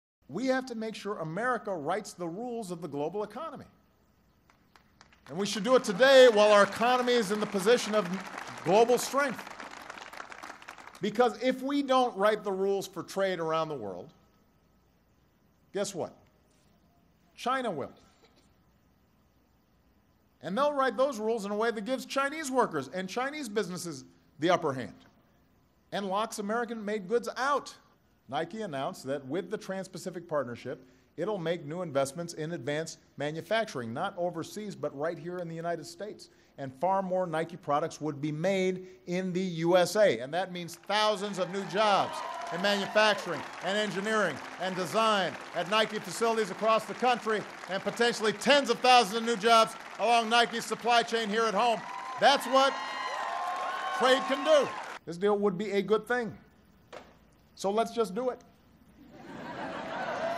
President Barack Obama visited Nike headquarters to promote his trade agenda. Nike says a Trans-Paci
(8 May 2015) FOR CLEAN VERSION SEE STORY NUMBER: apus030935

President Barack Obama argued forcefully Friday for a pending international trade agreement, saying the U.S. must write the rules for global commerce now, while it's in a position of economic strength, or lose out.
"If we don't write the rules for trade around the world, guess what? China will," Obama said, making his case at the Oregon headquarters of the athletic apparel and footwear company Nike. "And they'll write those rules in a way that gives Chinese workers and Chinese businesses the upper hand."
Nike was a curious choice for the president's speech, given criticism of its labor practices over the years.
But the company pledged Friday to create up to 10,000 jobs in the U.S. over the next decade with its manufacturing partners if Congress gives Obama "fast-track" trade negotiating authority that ultimately leads to the 12-country Trans-Pacific trade deal.
Nike said the trade deal would allow it to take advantage of lower tariffs on shoes and speed up investments in footwear manufacturing in the U.S.
Negotiations over the deal have put Obama in the awkward position of publicly battling with Democratic allies who normally would support him, including many lawmakers, labor unions and environmentalists.
Opponents fear the deal would lead to jobs being shifted overseas.
Obama argued that the current system doesn't benefit American workers, particularly those in the small businesses that he said create two out of every three jobs in the U.S. He said those businesses need easier access to the fast-growing Asian markets.